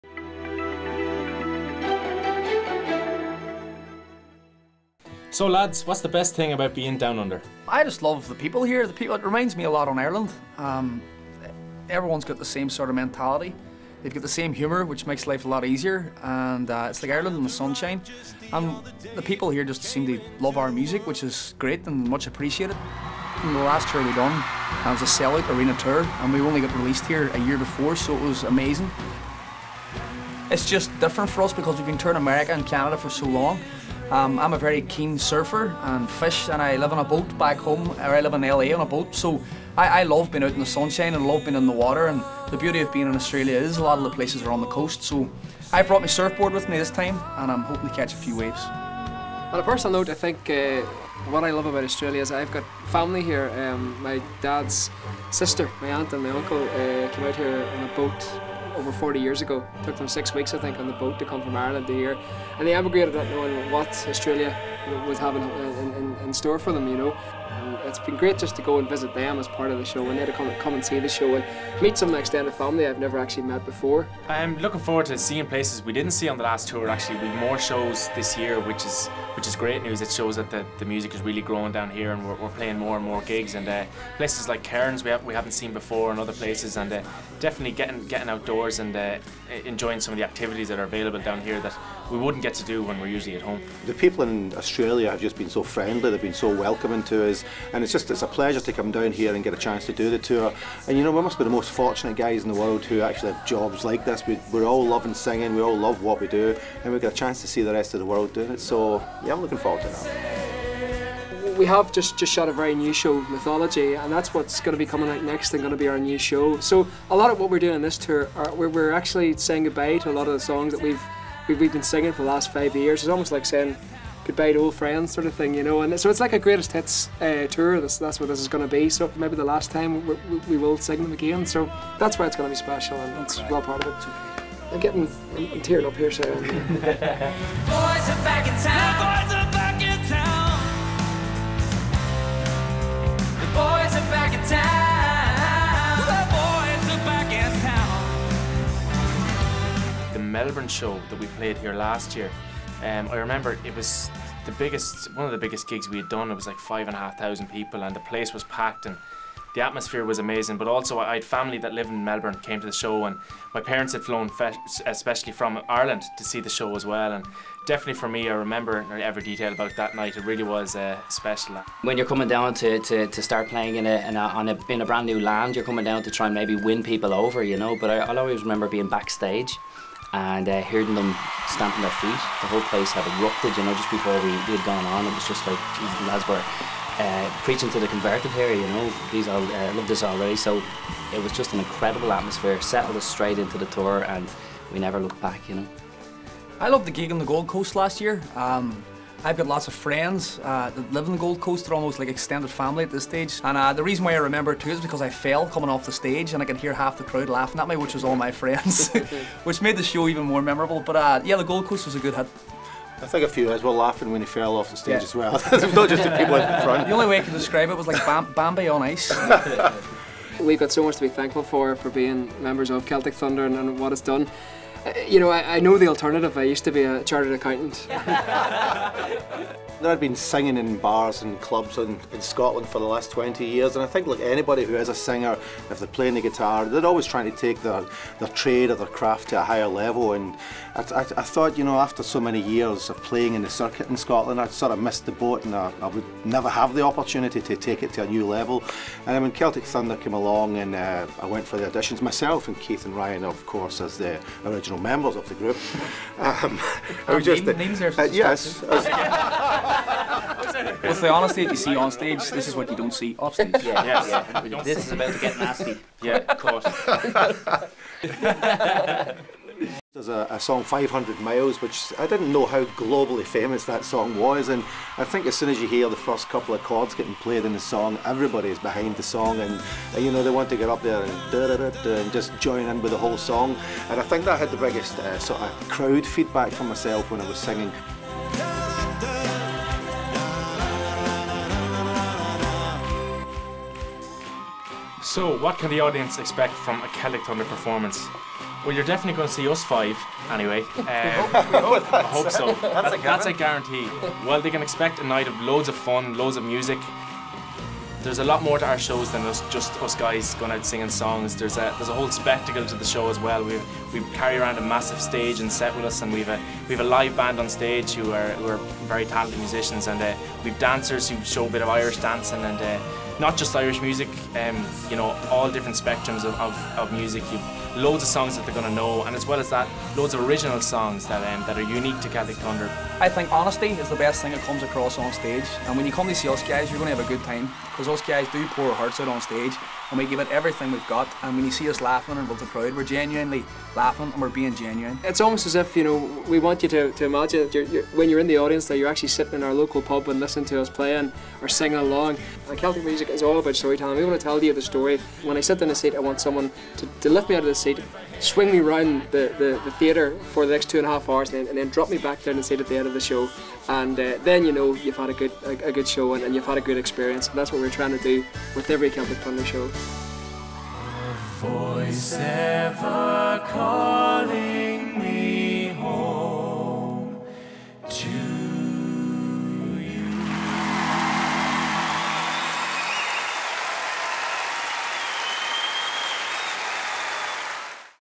Celtic Thunder Interview Exclusive
Join us as we sit down with the boys from Celtic Thunder on their most recent trip to Sydney.